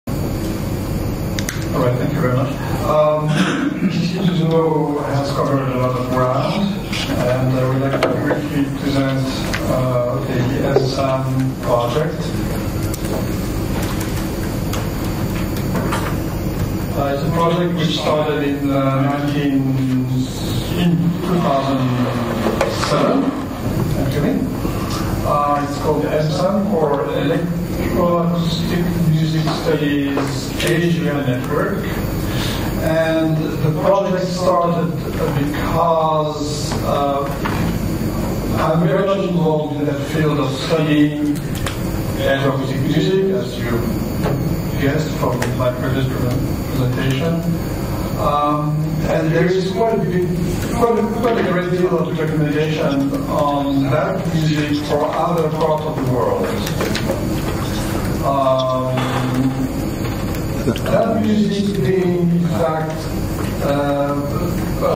第12回JSSA研究会6件目Battierさん講演その1
Marc Battier
Databases devoted to electroacoustic music studies in East Asia: the EMSAN databases